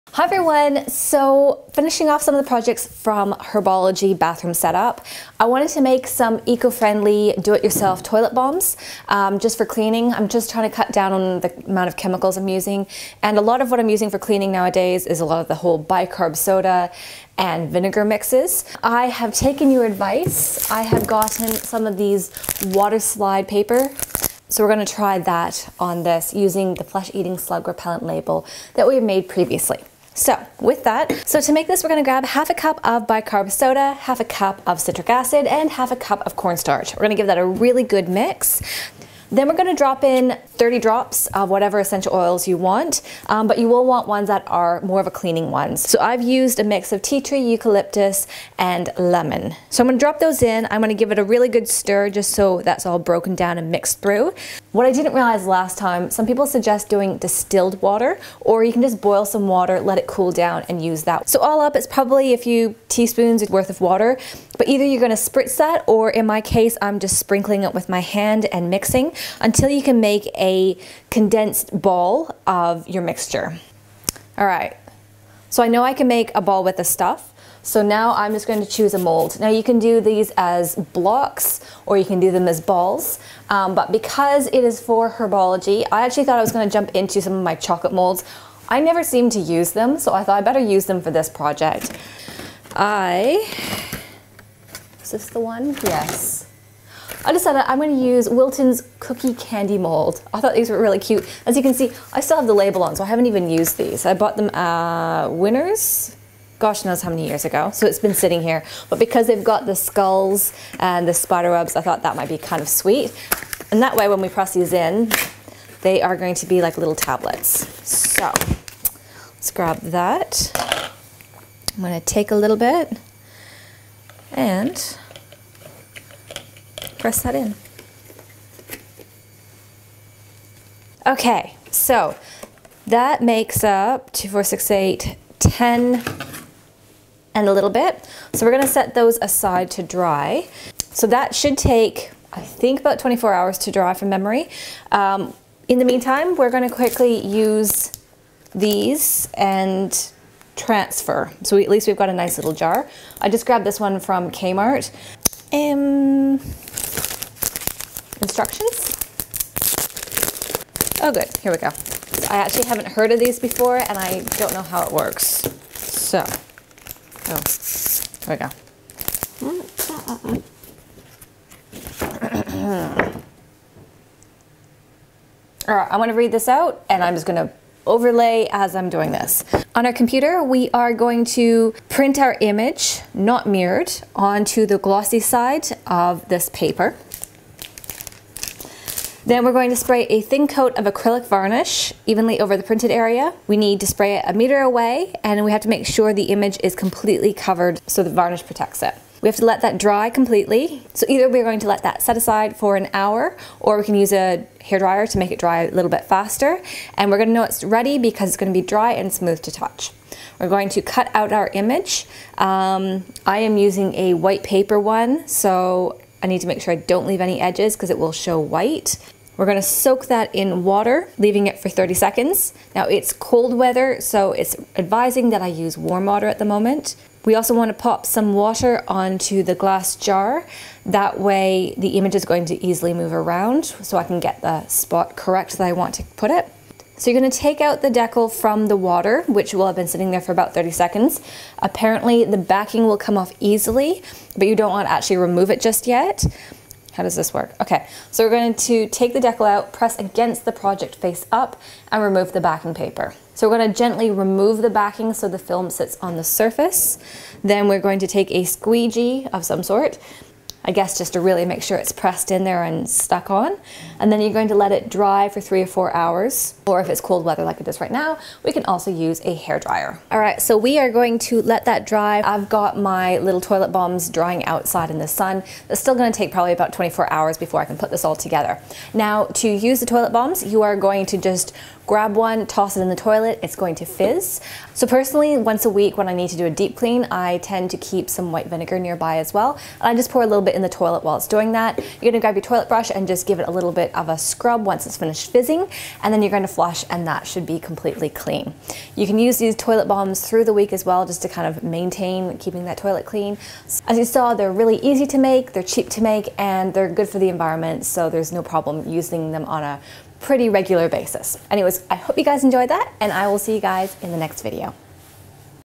Hogwarts Herbology DIY Eco Toilet Bombs | In Literature
Just like bath bombs, but for toilets 😀 Bicarb soda, citric acid and essential oils that clean/ disinfect make up these eco-friendly toilet bomb cleaners. I've packaged ours to match the rest of the Hogwarts Herbology inspired bathroom we have going on.
The waterslide decal paper is also available in clear
And you'll find the Hogwarts Herbology labels are all available in this blog post.